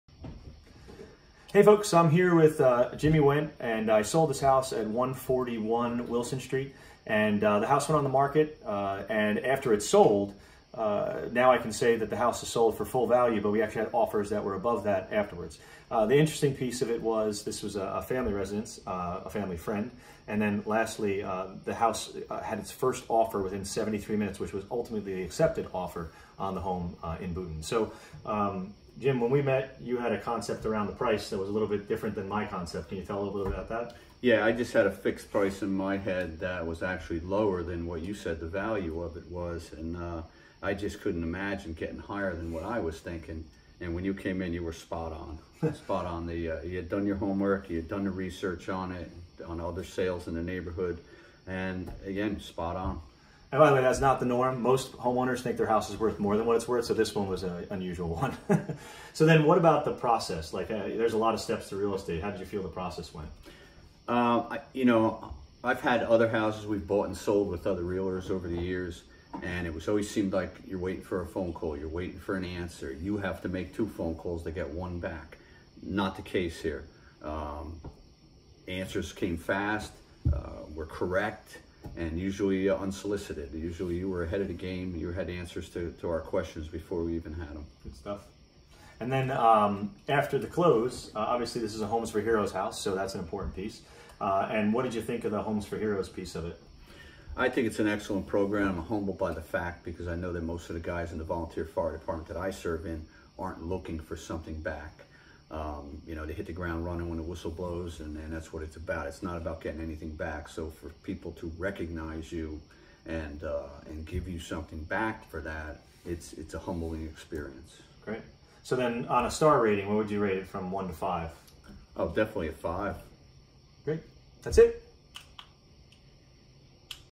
Homes for Heroes - Firefighter Hero Testimonial
Served this Firefighter Heroe, sold their family home.  First offer in 73mins and over asking offers.